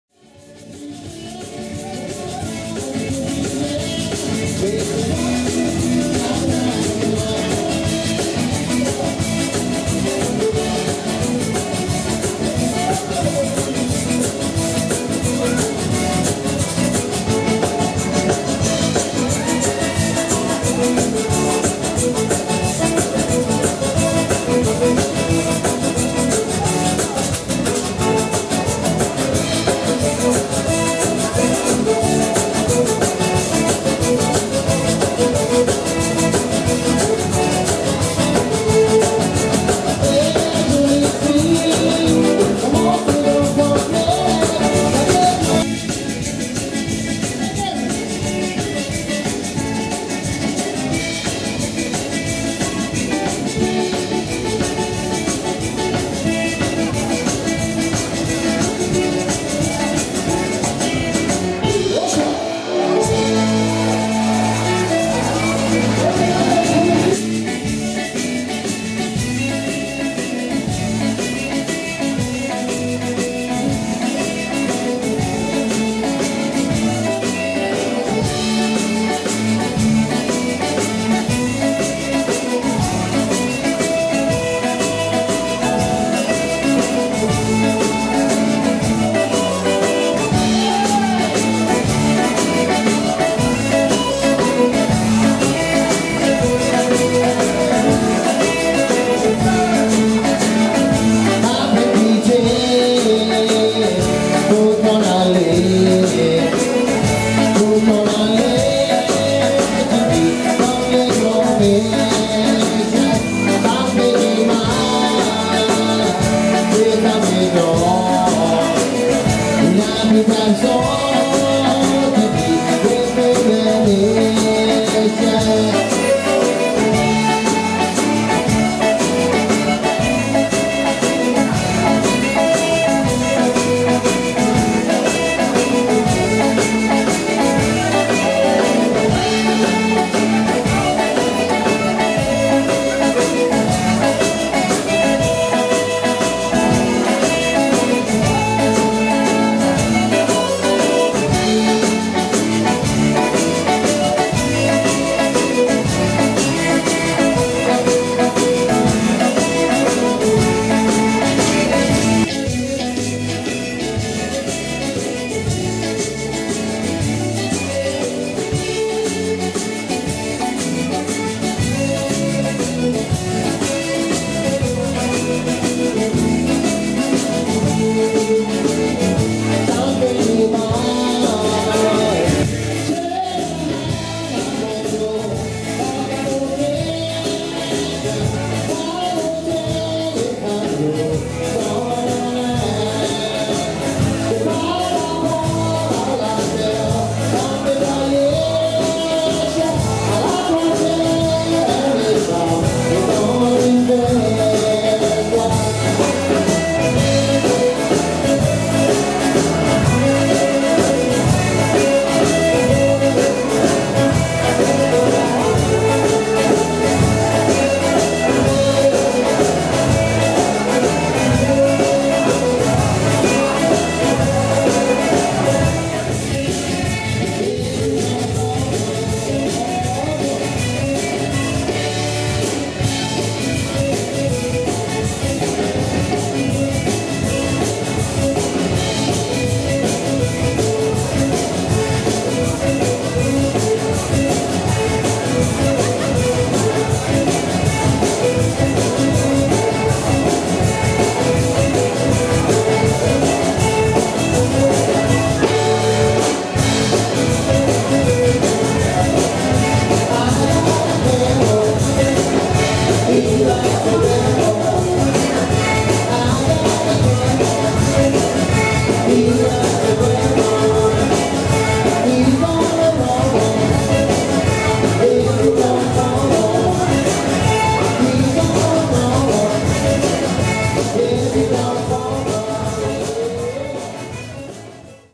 New Year’s Eve and dancing to Geno Delafose
New Year’s Eve and dancing to Geno Delafose at Vermilionville on 12/31/16. For high definition, on a computer put cursor on the "gear" symbol and then select on “1080”.